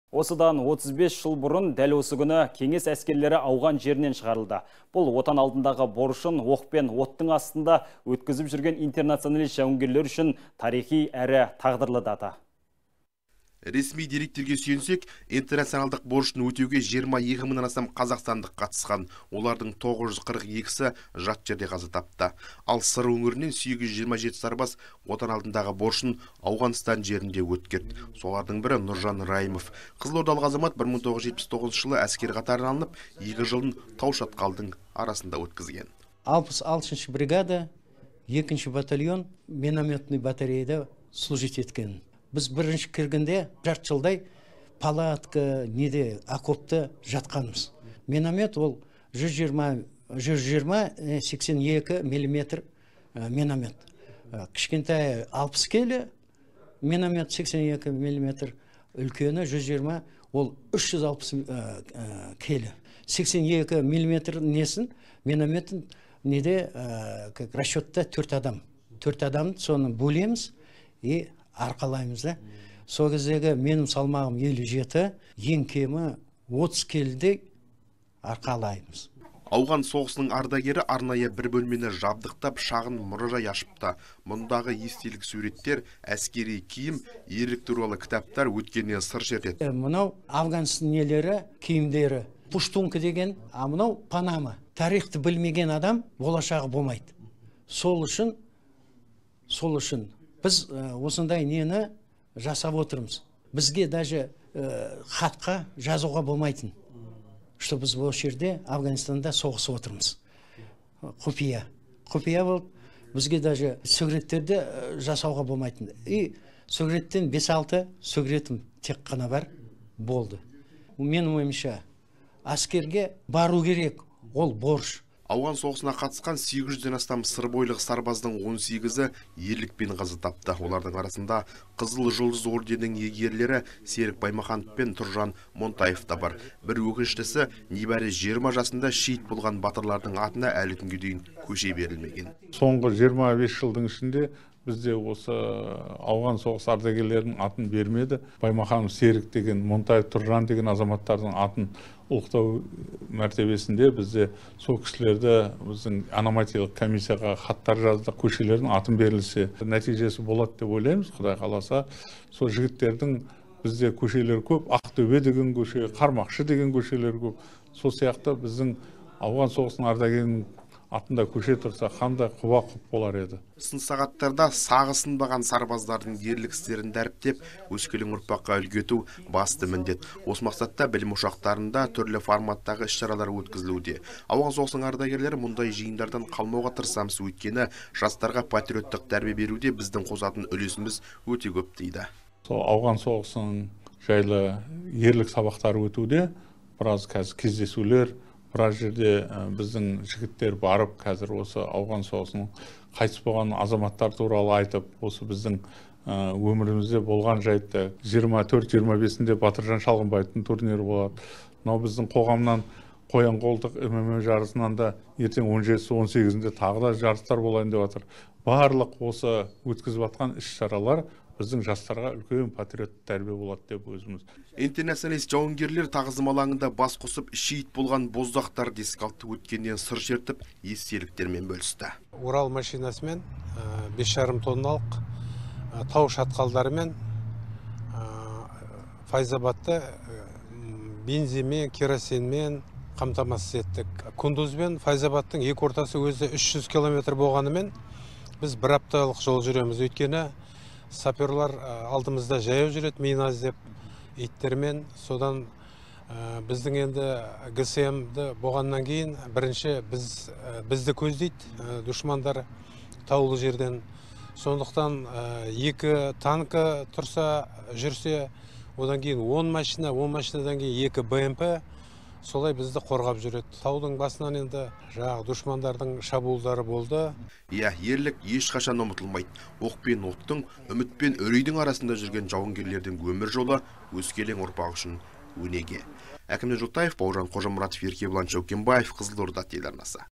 Тарихи әрі тағдырлы дата
Осыдан 35 жыл бұрын дәл осы күні Кеңес әскерлері Ауған жерінен шығарылды. Бұл Отан алдындағы борышын оқ пен оттың ортасында өткізген интернационалист жауынгерлер үшін тарихи әрі тағдырлы дата.

Ресми деректерге сүйенсек, интернационалдық борышын өтеуге 22 мыңнан астам қазақстандық қатысқан. Олардың 942-і жат жерде қаза тапты.  Ал, Сыр өңірінен 827 сарбаз Отан алдындағы борышын Ауғанстан жерінде өткерді. Солардың бірі – Нұржан Райымов. Қызылордалық азамат 1979 жылы әскер қатарына алынып, екі жылын тау-шатқалдың арасында өткізген.

НҰРЖАН РАЙЫМОВ - АУҒАН СОҒЫСЫНЫҢ АРДАГЕРІ //66 бригада, 2 батальонда минаметші болып служить еткем. Бірінші кіргенде ешқандай казарма болған жоқ. Жарты жылдай палаткада, окопта жаттық. Минамет 120,82 мм болады. Кішкентайы 60 келі, үлкені 360 келі. //

Ауған соғысының ардагері арнайы бір бөлмені жабдықтап, шағын мұражай ашыпты. Мұндағы естелік суреттер, әскери киім, ерлік туралы кітаптар өткеннен сыр шертеді әрі өскелең ұрпақ үшін өнеге.

НҰРЖАН РАЙЫМОВ - АУҒАН СОҒЫСЫНЫҢ АРДАГЕРІ //Мынау авганстың киімдері пуштунка деген, ал мынау панама. Тарихты білмеген адамның болашағы болмайды. Сол үшін жасап отырмыз. .Менің ойымша әскерге бару керек. Ол – борыш\

Ауған соғысына қатысқан сегіз жүзден астам сырбойылық сарбаздың 18-і ерлікпен қаза тапты. Олардың арасында «Қызыл жұлдыз» орденінің иегерлері Серік Баймаханов пен Тұржан Монтаев та бар. Бір өкініштісі, небәрі 20 жасында шейіт болған батырлардың атына әлі күнге дейін көше берілмеген.

ӘЛІБЕК ЖАРОВ   - «АУҒАН СОҒЫСЫНЫҢ АРДАГЕРЛЕРІ МЕН МҮГЕДЕКТЕРІНІҢ КЕҢЕСІ» ҚБ ТӨРАҒАСЫ //Соңғы 25 жылдың ішінде ауған соғысының ардагерлерінің атын бермеді. Серік Баймаханов пен Тұржан Монтаев деген азаматтардың атын ұлықтау мақсатында ономастикалық комиссияға хат жаздық. Көшелерге аты берілсе. Нәтижесі болады деп ойлаймын

Сын сағаттарда сағы сынбаған сарбаздардың ерлік істерін дәріптеп, өскелең ұрпаққа үлгі ету - басты міндет. Осы мақсатта білім ошақтарына түрлі форматтағы іс шаралар өткізілуде. Ауған соғысының ардагерлері мұндай жиындардан қалмауға тырысамыз. Өйткені жастарға патриоттық тәрбие беруде біздің қосатын үлесіміз өте көп,- дейді.

Интернационалист жауынгерлер Тағзым алаңында бас қосып, шейіт болған боздақтарды еске алды. Өткеннен сыр шертіп, естеліктерімен бөлісті.
Иә, ерлік ешқашан ұмытылмайды. Оқ пен оттың, үміт пен үрейдің арасында жүрген жауынгерлердің өмір жолы өскелең ұрпақ үшін өнеге.